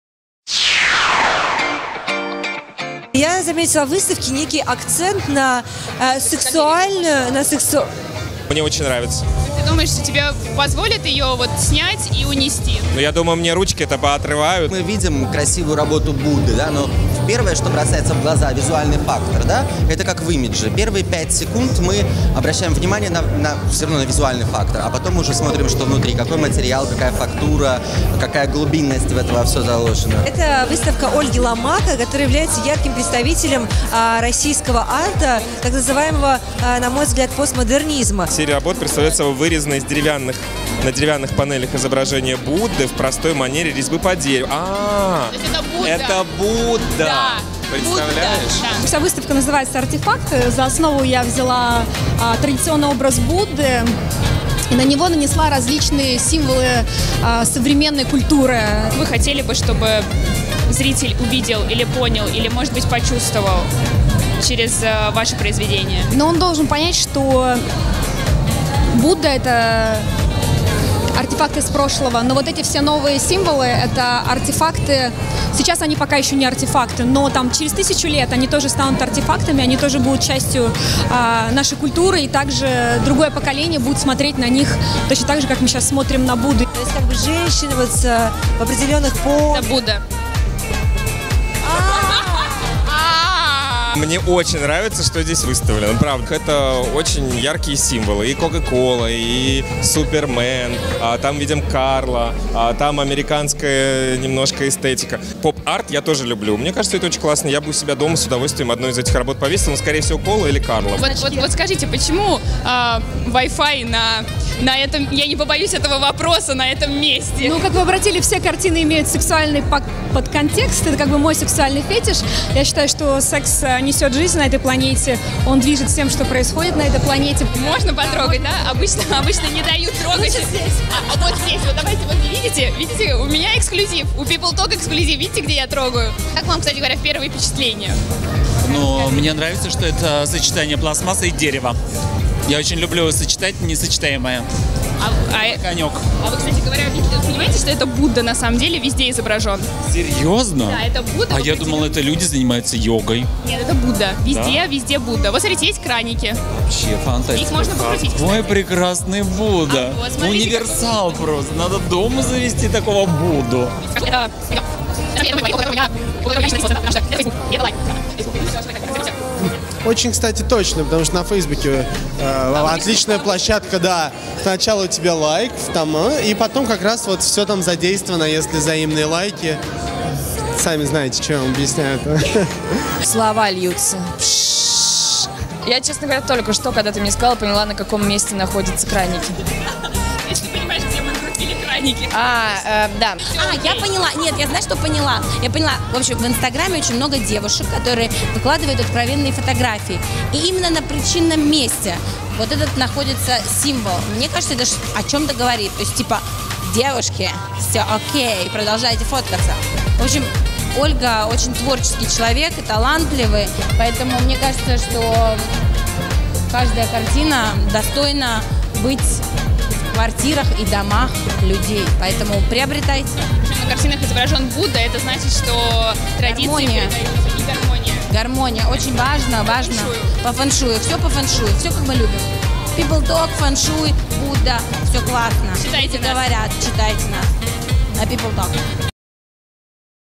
PEOPLETALK Выставка художника Ольги Ломака
18 февраля особняк Спиридонова на один вечер превратился в модное концептуальное арт-пространство благодаря закрытой презентации нового проекта российско-британского художника Ольги Ломака. Мероприятие прошло при поддержке журнала Harper’s Bazaar Art, а организатором вечера выступило коммуникационное агентство FashionPRBuro.

Поддержать и поздравить Ольгу с презентацией пришли: куратор и искусствовед Василий Церетели, Александр Юдин, финансист и меценат Стюарт Лоусон. Марианна Максимовская,художник Андрей Бартенев, fashion-эксперт Олег Тарнопольский, Анна Русска, Екатерина Лиепа, актрисы Ингрид Олеринская, Мария Шумакова, Ксения Князева, Дарья Авратинская и Наталья Бычкова.

Также этот светский арт-вечер посетили Константин Андрикопулос и Ольга Цыпкина, Лаура Джугелия, Ольга Новожилова, Александра Михалкова, телеведущий Артем Королев, стилист Александр Рогов, Милана Королева, Дарья Михалкова, Виталий Козак, дизайнеры Константин Гайдай, Александр Сирадекиан и Диана Джанелли.

Представители культурной элиты и московский свет знакомились с экспозицией под DJ сет Александры Федоровой, наслаждаясь контрастной атмосферой поп-арта и классической русской архитектуры. Оценить работы Ольги в этот вечер пришли и профессионалы искусствоведы, галеристы, кураторы, арт-критики и представители аукционного дома Phillips.

Ольга Ломака известна в культурной среде не только своими яркими работами, несущими в себе явную толику провокации и искрометных намеков на проблематику бытия современного человека, но и уникальной эпатажной подачей своего творческого видения на выставках, презентациях и закрытых превью. В этом году художник привезла в Москву серию работ «Артефакты», устроив специальный арт-вечер, не разочаровав поклонников современного искусства в России.
Экспозиция будет представлена с 22 февраля по 4 марта в галерее 25 Kadr.